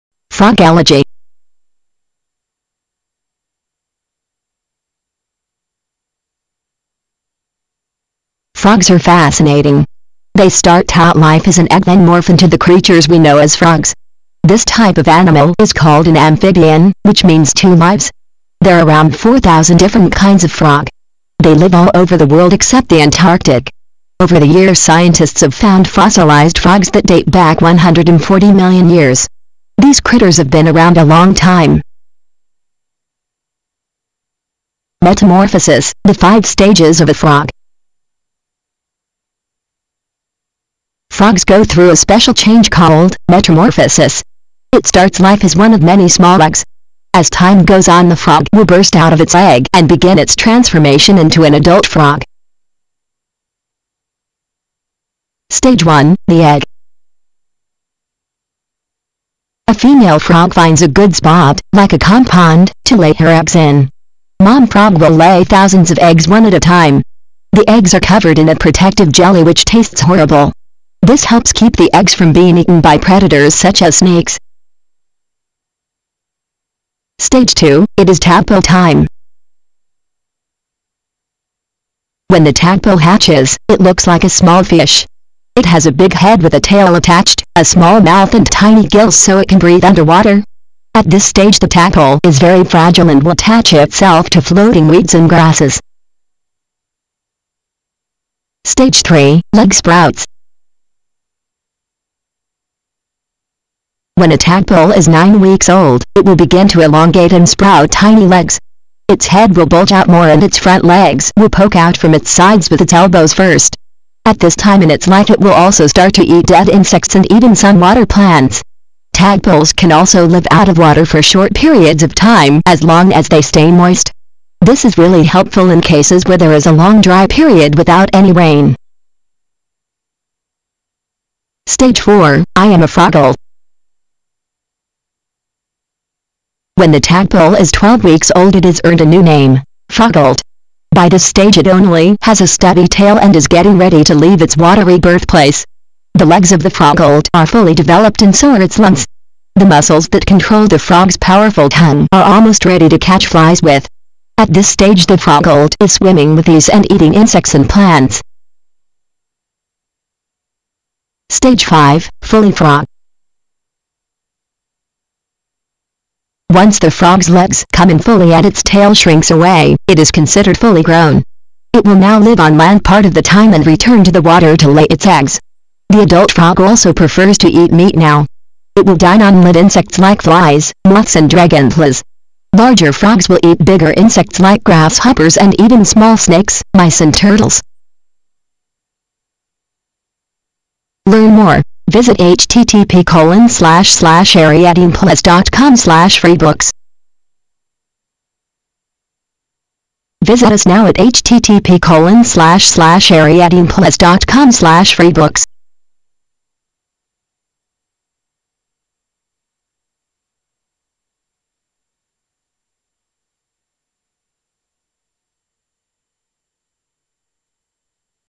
FROG-OLOGY
FROG-OLOGY. A Kids video book about frogs. Learn How to get free kindle Kids books directly from Amazon This is adapted from the book: Children's book on frogs: A Kids Picture book about frogs with Photos and fun Facts. This video is filled with about 18 wonderful images and plenty of fun facts for kids of all ages 3-12. Enjoy.